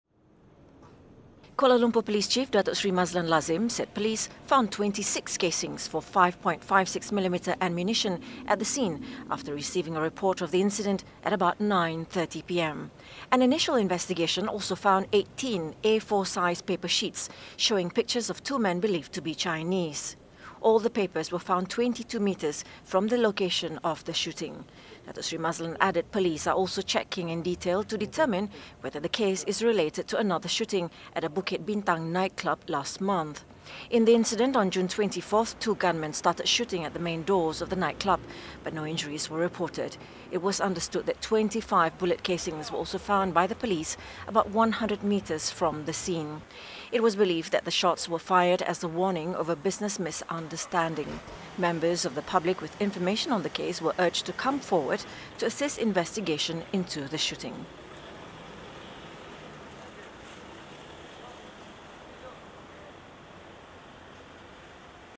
Another Shooting Incident At KL Nightspot
This evening there was a shooting incident at an entertainment centre in Taman Maluri, Cheras.

It was believed that at least 26 shots were fired from an M-16 rifle outside the premises last night but luckily, there were no injuries reported.